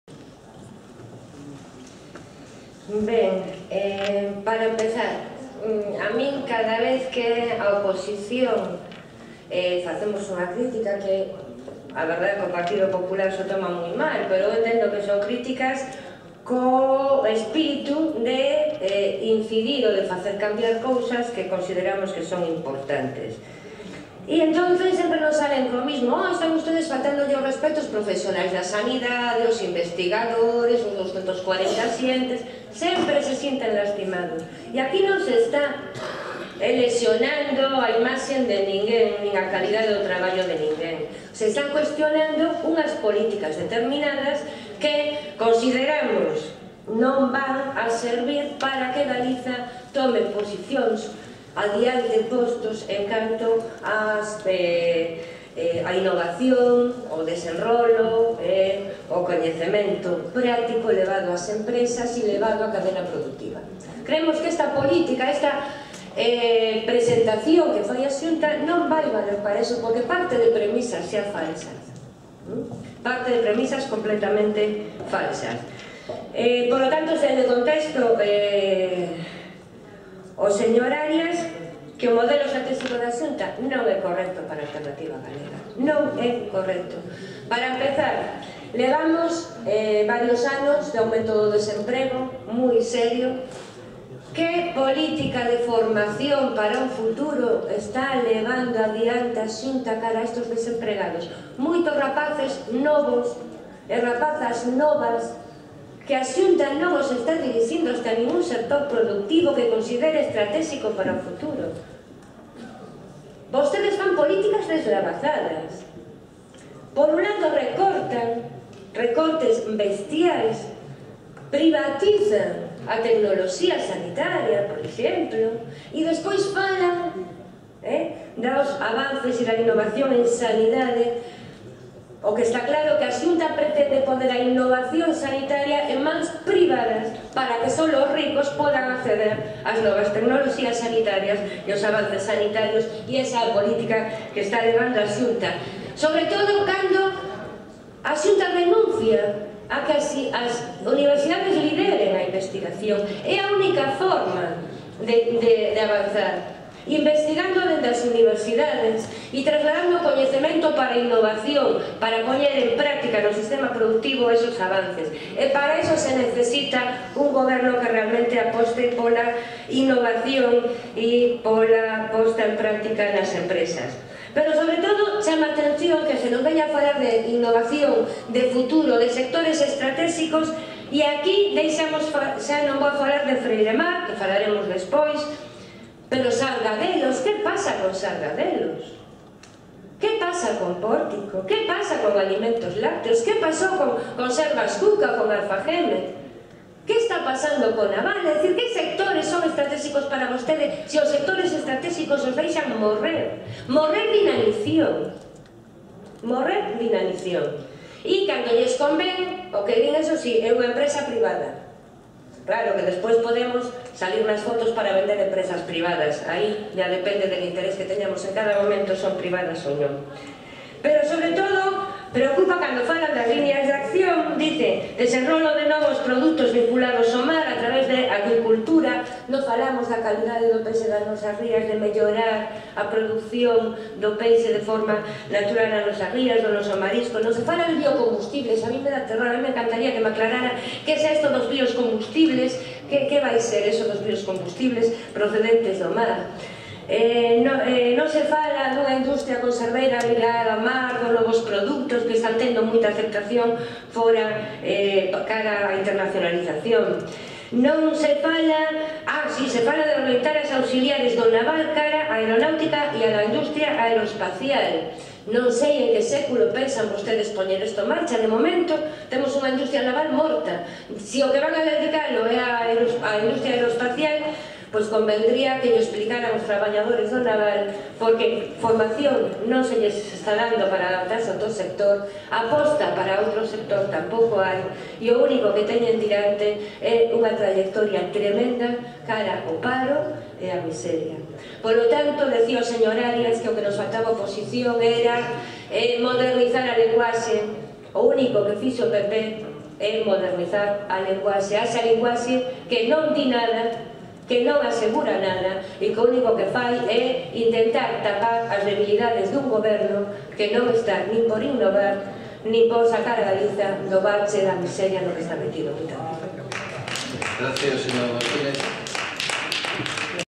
Segunda intervención de Consuelo Martínez na comparecencia do conselleiro de Economía
Segunda intervención da deputada de AGE, Consuelo Martínez, na comparecencia do conselleiro de Economía sobre o desenvolvemento en Galicia da estratexia europea.